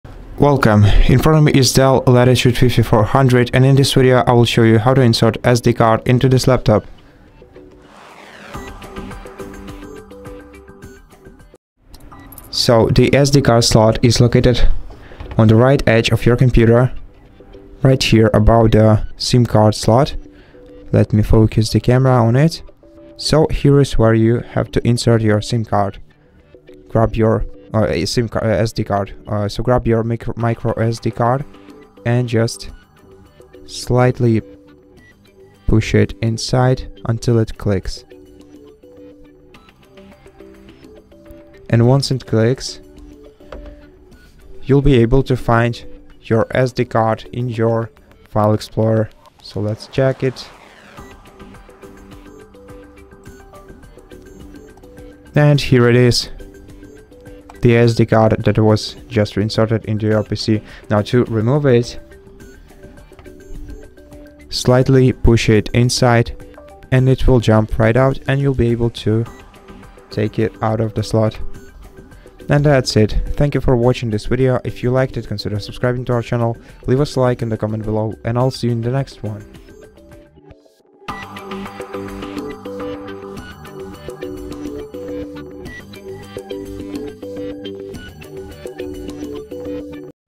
DELL LATITUDE 5400 – How to Insert SD Card
If you want to expand your storage or transfer files on your DELL LATITUDE 5400, this video will show you exactly how to insert an SD Card into your laptop. We’ll guide you step by step, from locating the SD Card slot on the right edge of your DELL LATITUDE 5400 to properly inserting your micro SD Card until it clicks into place. You’ll also learn how to check if your SD Card is recognized in File Explorer and how to safely remove it when you’re done. This simple process makes it easy to add extra storage or move photos, videos, and documents between devices using your DELL LATITUDE 5400.

How to insert SD Card in DELL LATITUDE 5400?
Where is the SD Card slot on DELL LATITUDE 5400?
How to safely remove SD Card from DELL LATITUDE 5400?

0:00 Introduction
0:07 Locating the SD Card slot on DELL LATITUDE 5400
0:20 Inserting the SD Card
0:38 Checking SD Card in File Explorer
1:01 How to remove the SD Card
1:12 Outro